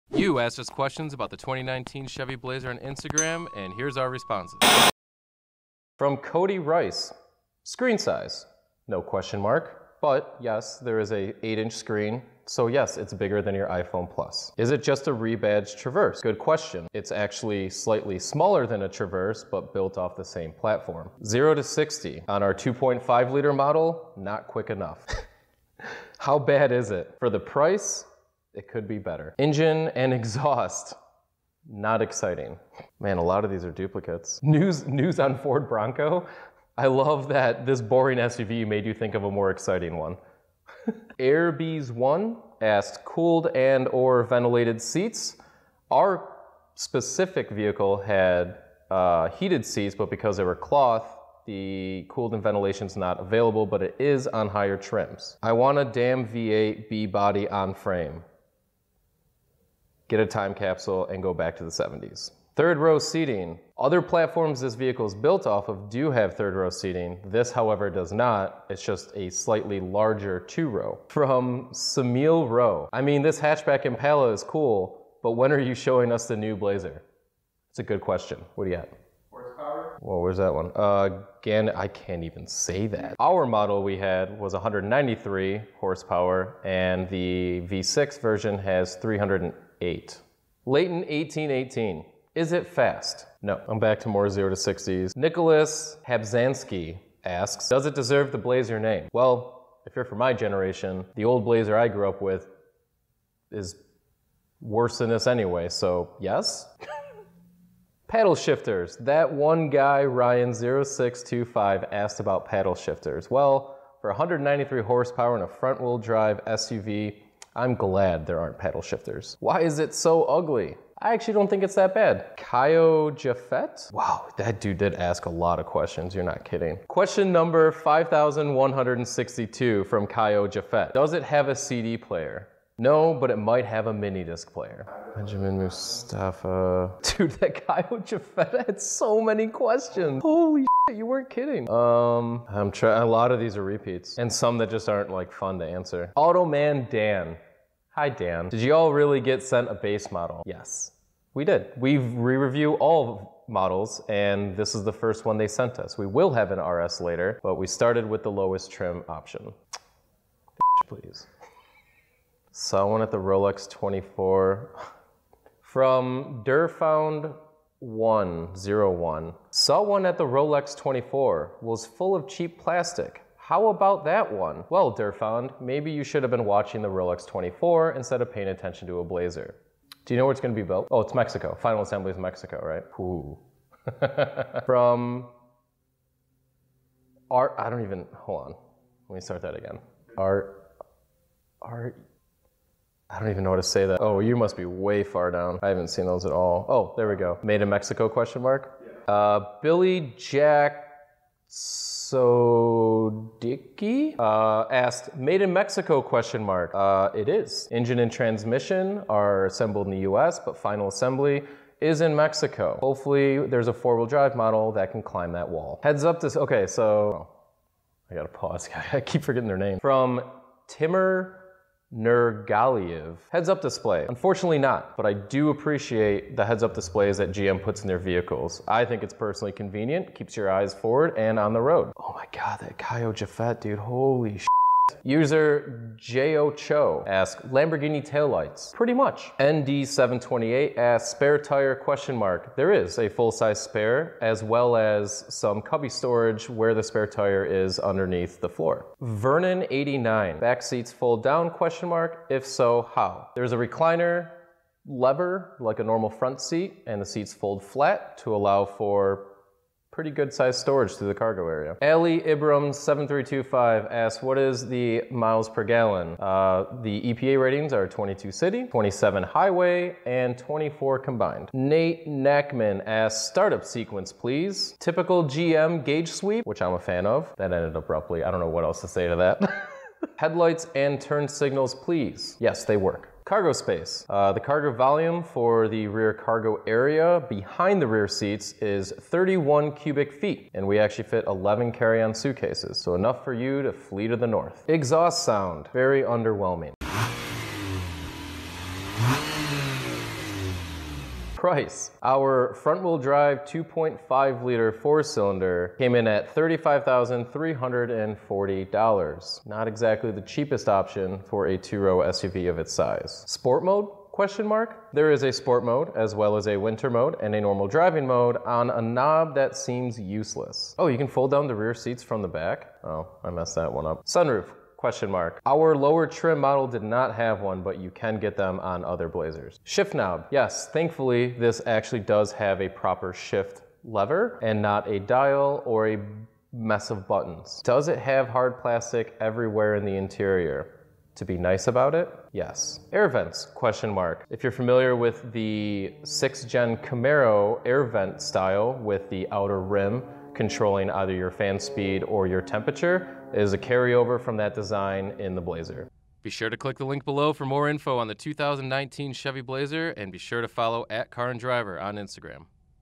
2019 Chevrolet Blazer: We Answer Your Questions
You asked questions on Instagram about the 2019 Chevrolet Blazer, and here are our answers.